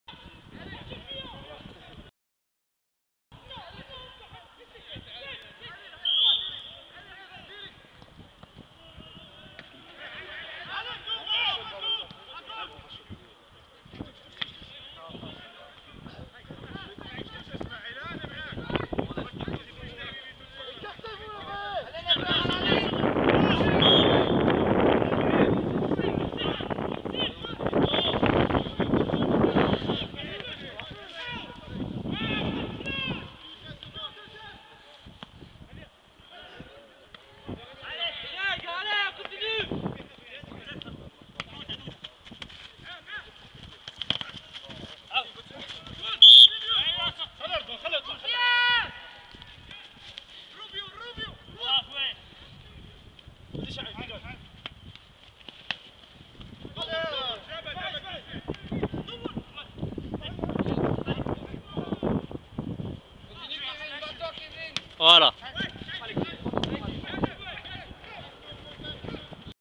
fcatlas1
c'est un match de foot de fc atlas une equipe qui joue en 4 divtion a charleroi c'est equipe maghrebin elle a gagne  dans cet match  a 4  - 0   . c'est le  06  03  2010 a goutrou  c'est les activites des maghribin en belgique le sport c'est un bon porte pour etre intigre dans  le monde belge merci a  rgcc

  merci a nos joueurs et a fouad et ayoure